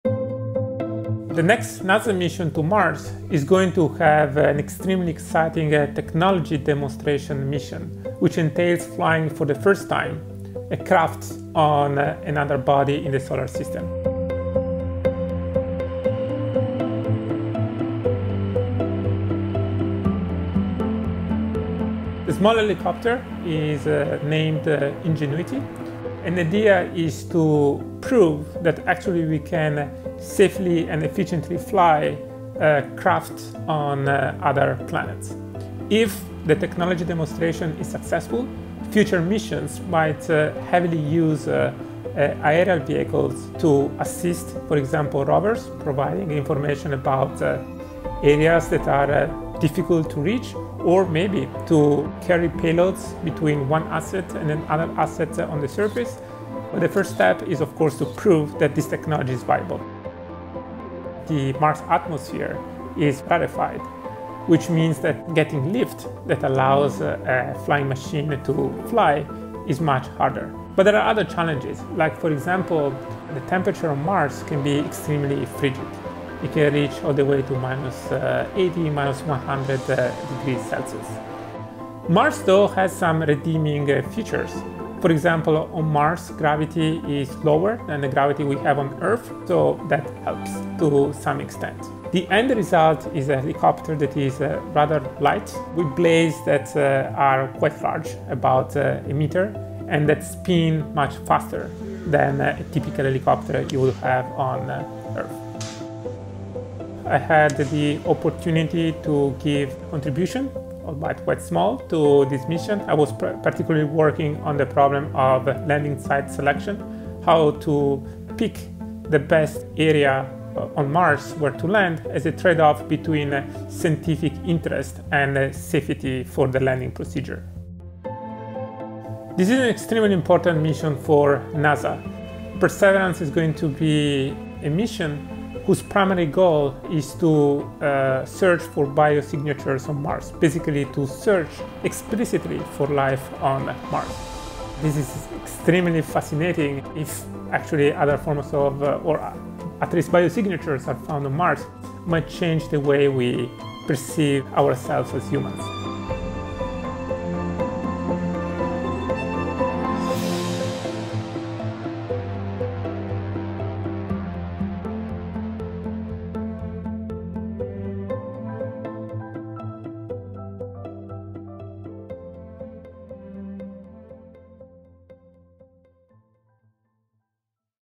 Drone copter to take flight for the first time on Mars
Despite a global pandemic and some technical delays, NASA’s 2020 Mars Rover – the aptly named Perseverance – is scheduled to launch this summer (currently slated for July 30) on a groundbreaking endeavor. Perseverance will bring along a small, four-pound helicopter dubbed Ingenuity. The copter will be the first flying vehicle on another planet and will have to navigate the frigid nights and dust-filled skies of Mars while operating in an atmosphere that is 100 times thinner than Earth’s. Associate Professor of Aeronautics and Astronautics
Marc Pavone played a role in determining the mission's landing site.

SYNC ID: MB01KYUH7PSPQLK